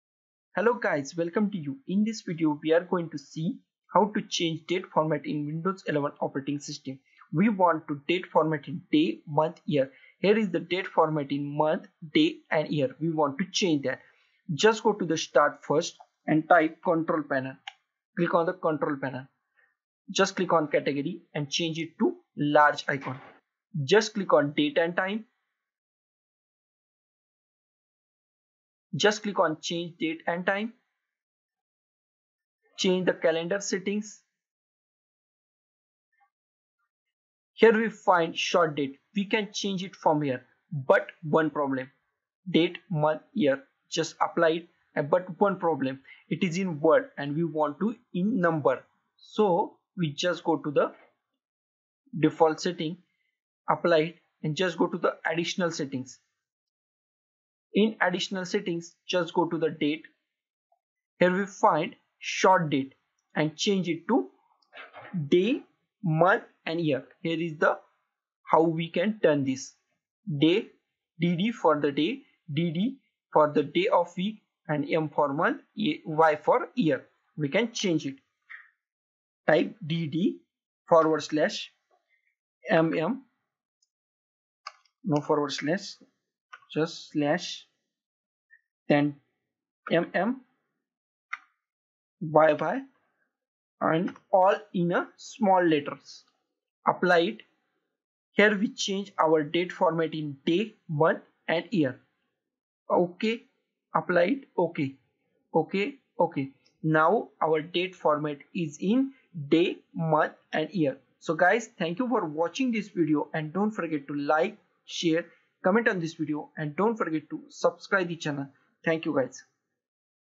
How to Change Date Format in Windows 11 - dd-mm-yyyy format
In this video we are going to see how to change date format in Windows operating system. by default Windows date format in month day and year format and we want to change it to day month and year format  by the simplest way.